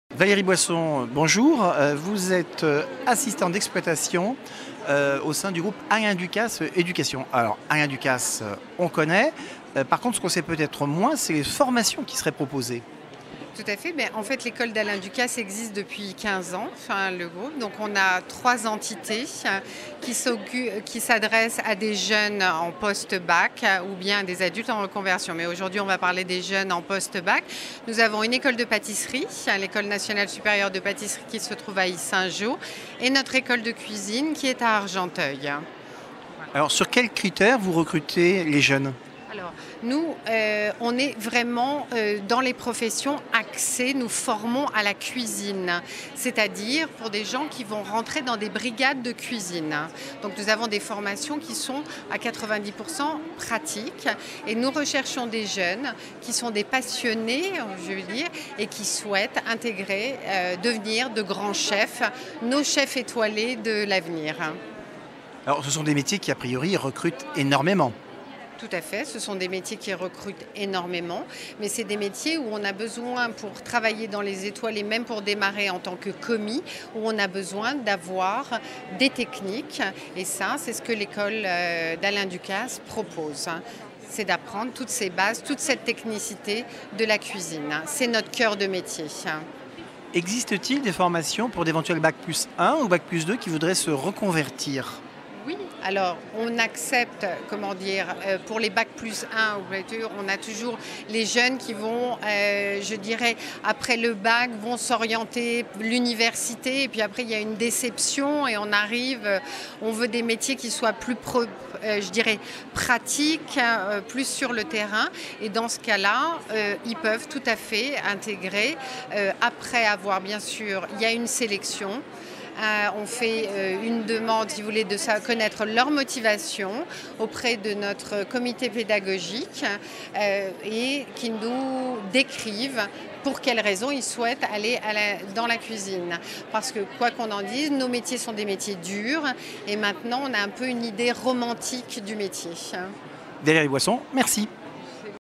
Futurs patissiers, cuisinisiers, Alain Ducasse Education propose des formations pratiques
Alain Ducasse Formation propose des formations pratique spour les futurs patissiers ou cuisiniers. Valérie Boisson, assistante d'exploitation Alain Ducasse Education, vous présente ces formations et leurs débouchés